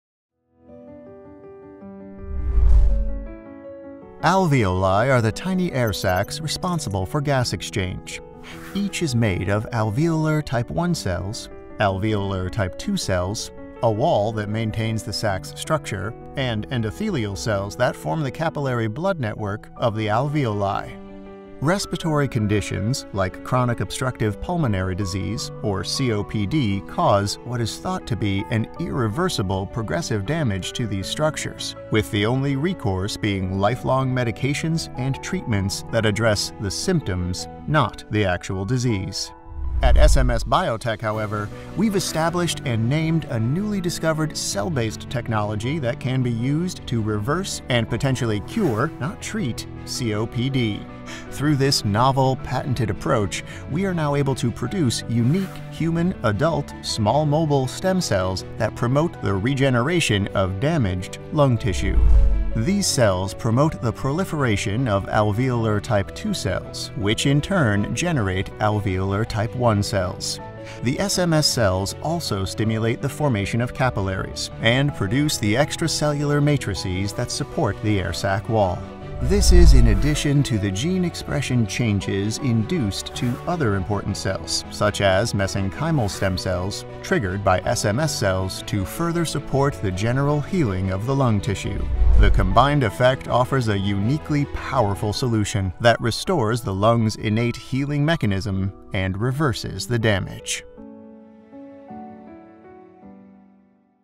Unveiling Hope: A Potential Breakthrough Treatment for COPD from SMSbiotech
In this eye-opening video, get a glimpse of the profound challenges faced by individuals battling Chronic Obstructive Pulmonary Disease (COPD) and unveil an innovative potential treatment that holds the promise of not only alleviating symptoms but potentially reversing and even curing this debilitating condition.

Understanding COPD: The Silent Struggle
COPD is a progressive lung disease that can significantly impact the quality of life for those affected. From breathlessness and chronic cough to fatigue and limited physical activity, the struggle is real. Join us as we shed light on the daily challenges faced by COPD patients and the urgent need for breakthrough solutions.

Our Revolutionary Approach: A Ray of Hope
Discover the groundbreaking potential treatment being developed by our dedicated team of scientists and advisors. We take pride in introducing a revolutionary approach aimed at not just managing COPD symptoms but potentially reversing the damage and offering a path toward a cure.

The Science Behind It All
Get introduced to the science behind our potential treatment as we explain how it works to address the root causes of COPD. We're committed to providing hope for COPD patients and their loved ones.

As we share our scientific breakthroughs with the world, we invite you to be a part of the movement to raise awareness about COPD and advocate for innovative treatments.

Together, we can make a difference in the lives of those affected by COPD. Join us on the journey to a future where COPD is not just managed but conquered.